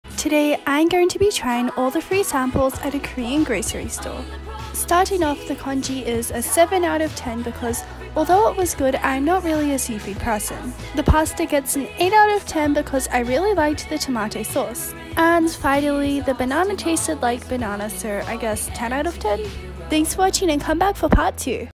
Pt. 1 Trying ALL the free samples at a Korean store: Travel food diary Korea pt.7
Part one of trying all the samples in a Korean Grocery store! (Don’t worry, I bought lots of food :)

My food diary: Korea is my diary of the food I eat in Korea.